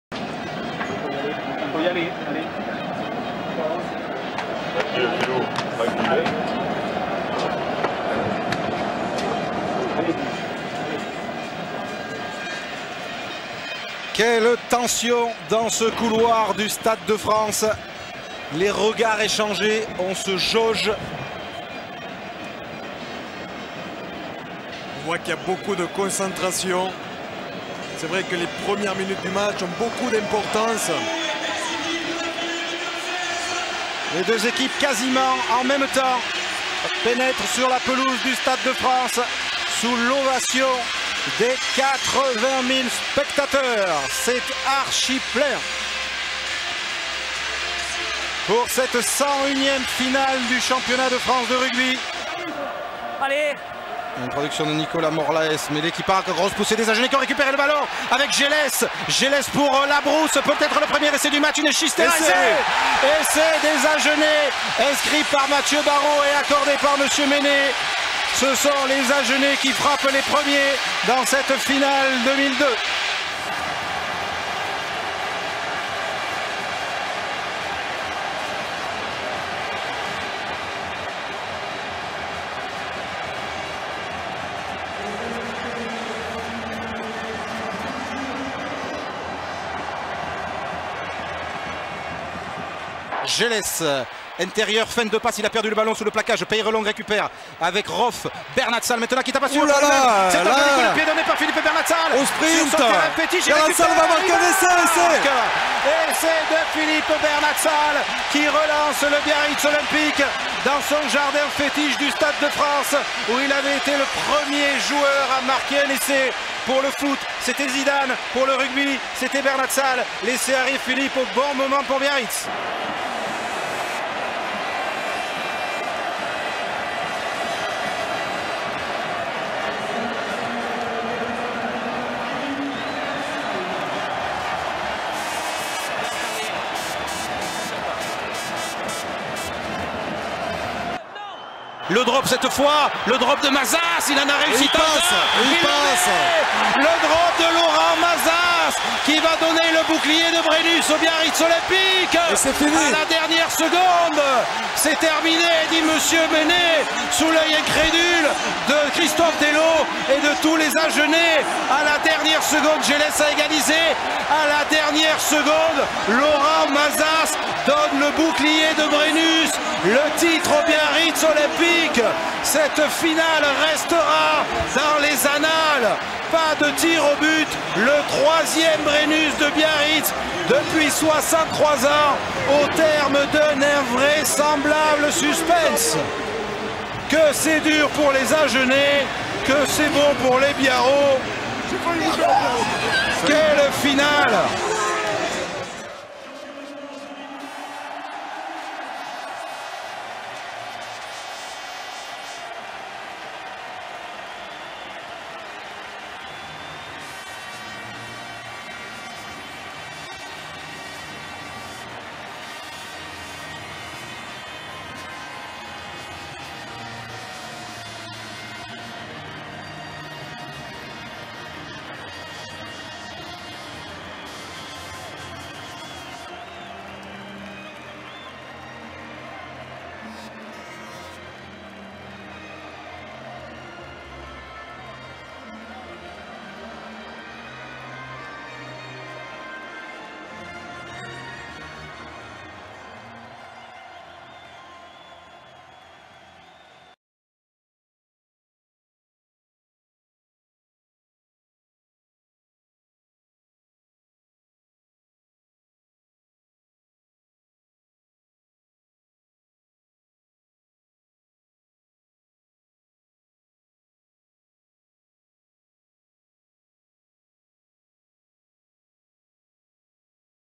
2001-2002 Résumé Finale Biarritz Olympique - SU Agen, championnat de France de rugby à XV
Match du 8 juin 2002 au Stade de France, Saint-Denis.

Arbitrage de Mr Didier Mené.

Composition du BO :
Titulaires : Emmanuel Ménieu, Jean-Michel Gonzalez, Denis Avril, Jean-Philippe Versailles, Olivier Roumat, Serge Betsen, Christophe Milhères, Thomas Lièvremont, Nicolas Morlaes, Julien Peyrelongue, Philippe Bidabé, John Isaac, Joe Roff, Philippe Bernat-Salles, Nicolas Brusque
Remplaçants : Olivier Nauroy, Didier Chouchan, Sotele Puleoto, Laurent Mazas, Guillaume Bousses, Noël Curnier, Stuart Legg

Composition du SUA :
Titulaires : Jean-Jacques Crenca, Jean-Baptiste Rué, Omar Hasan, David Couzinet, Christophe Porcu, Matthieu Lièvremont, Philippe Benetton, Thierry Labrousse, Mathieu Barrau, François Gelez, Christophe Manas, Luc Lafforgue, Conrad Stoltz, Pépito Elhorga, Christophe Lamaison
Remplaçants : Guillaume Bouic, Maurice Barragué, Nicolas Martin, Philippe Piacentini, Patrick Blanco, Xavier Guillemet, François Tandonnet